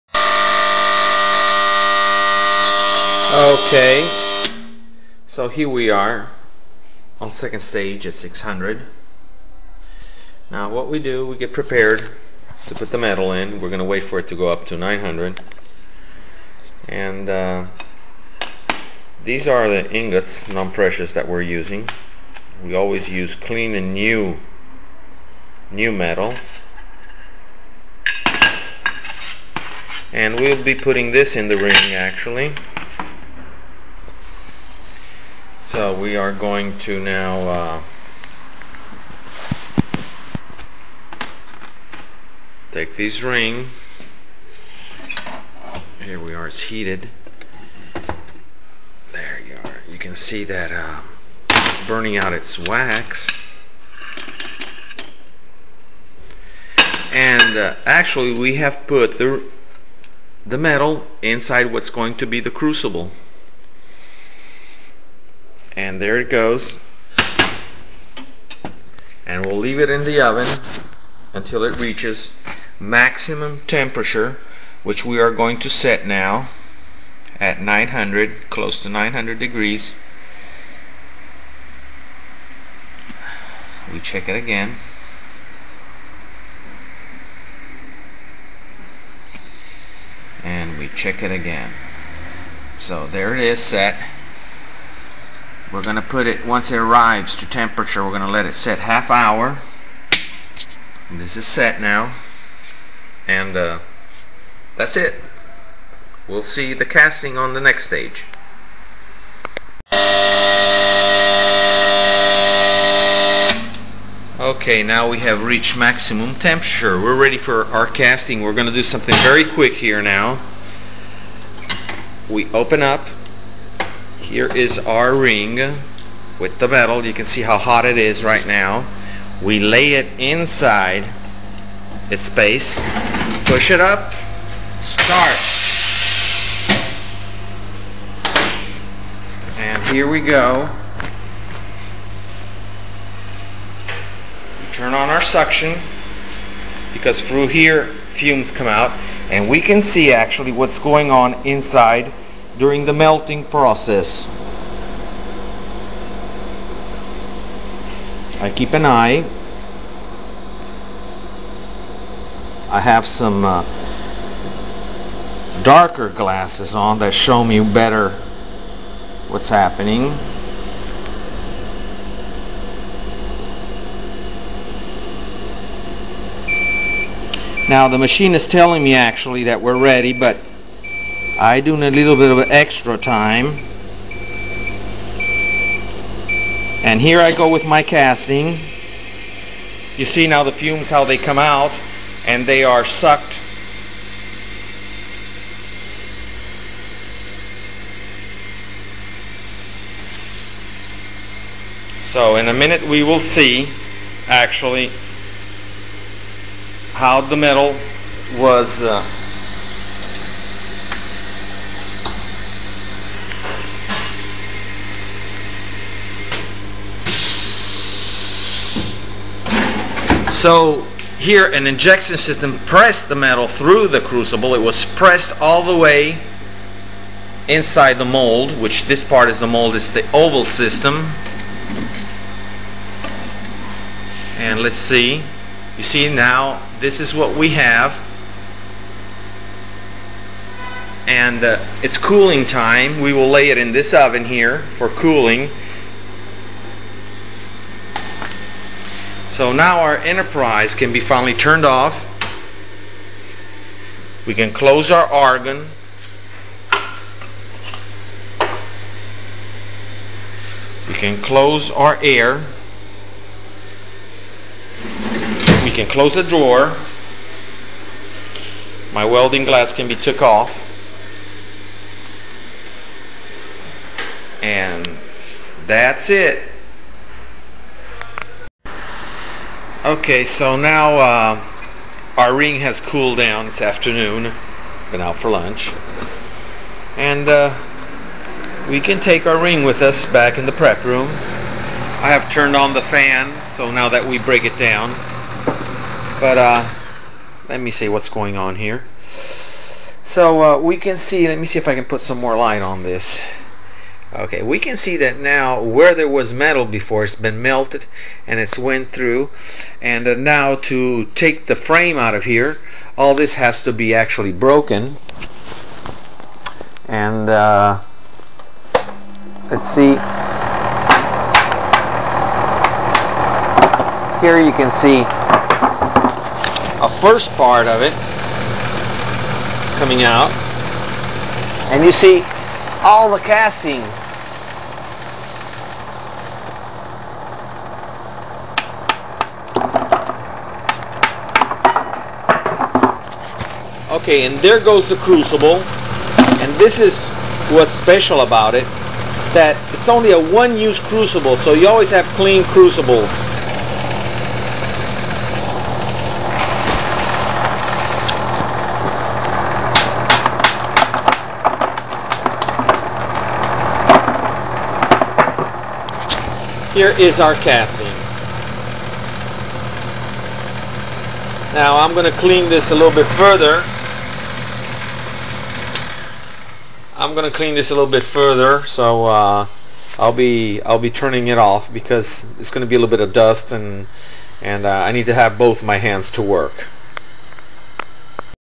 Edident Lab "Casting a 7 unit bridge Np Metal" part3
Edident's unique self-made Oval-Ringless-MonoCrucible technique for existing Enterprise Argon Induction equipment therefore casting NP Metals or any kind of Dental metals maintaining extreme precision & control over investment expansion and Metal cooling contraction